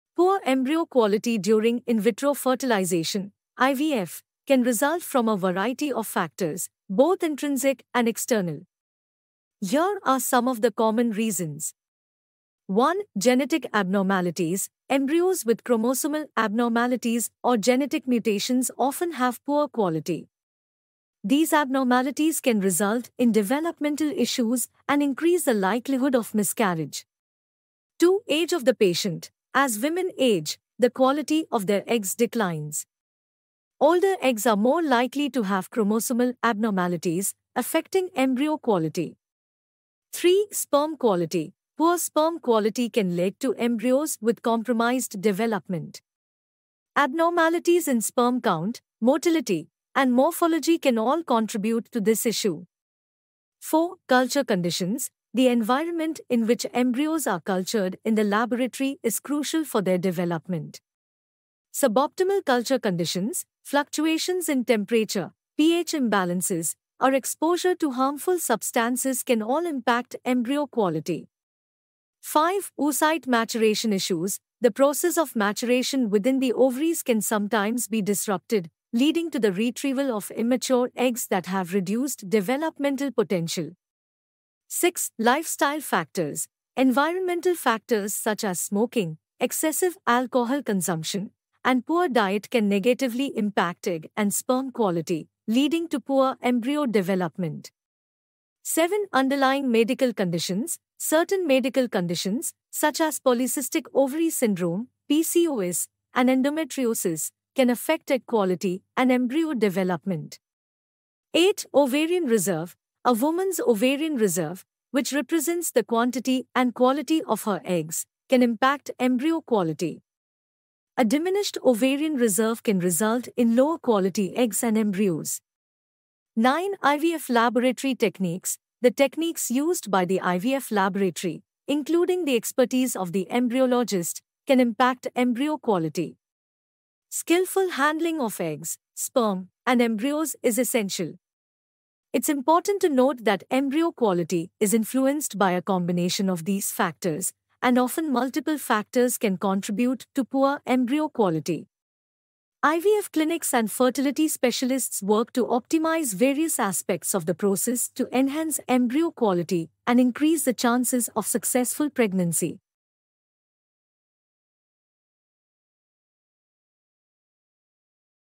Beyond the Microscope: Unveiling 9 Potential Reasons for Poor Embryo Quality in IVF 🔍❌
If you're on the IVF journey and have encountered challenges related to embryo quality, this video is for you. We delve into a crucial topic: "9 Reasons for Poor Embryo Quality in IVF." Join us as we explore the potential factors that could contribute to poor embryo quality during IVF. We discuss these reasons in detail, shedding light on the science behind each one and their impact on your fertility journey.